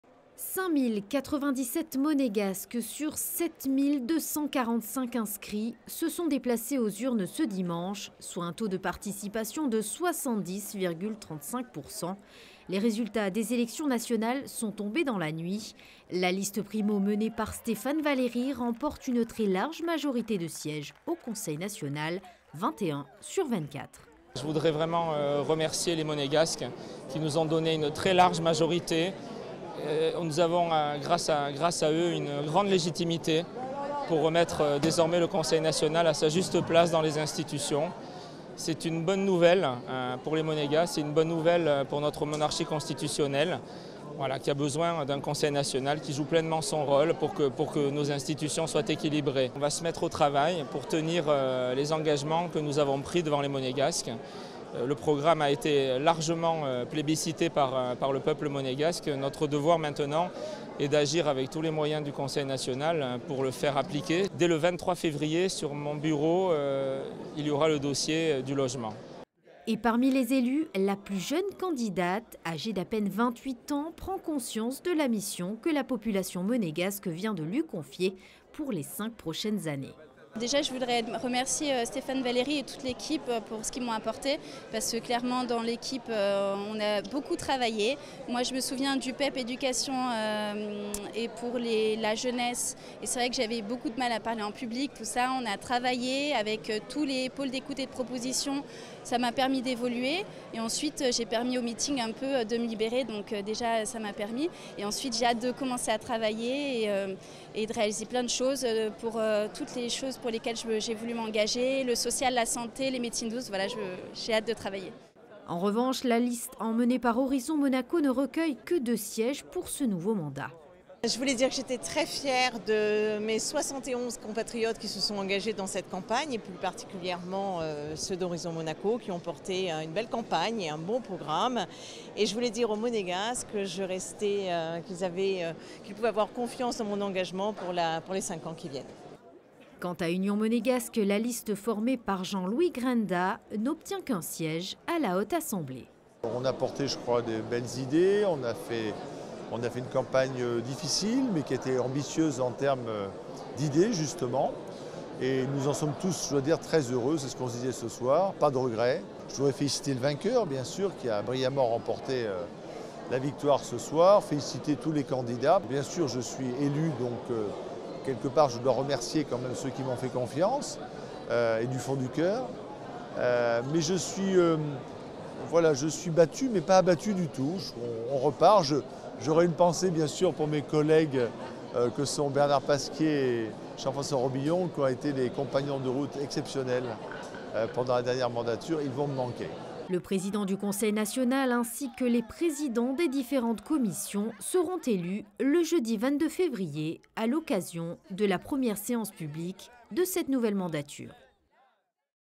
Les Élections Nationales de 2018
5097 Monégasques sur 7245 inscrits se sont déplacés dimanche dernier pour les élections nationales 2018, ce qui représente un taux de participation de 70,35%. La liste Primo, menée par Stéphane Valeri, a remporté une très large majorité de sièges - 21 sur 24 - au Conseil National de Monaco. La première séance publique de cette nouvelle mandature aura lieu le jeudi 22 février avec au programme : l’élection du Président du Conseil National de la Principauté ainsi que les Présidents des différentes Commissions.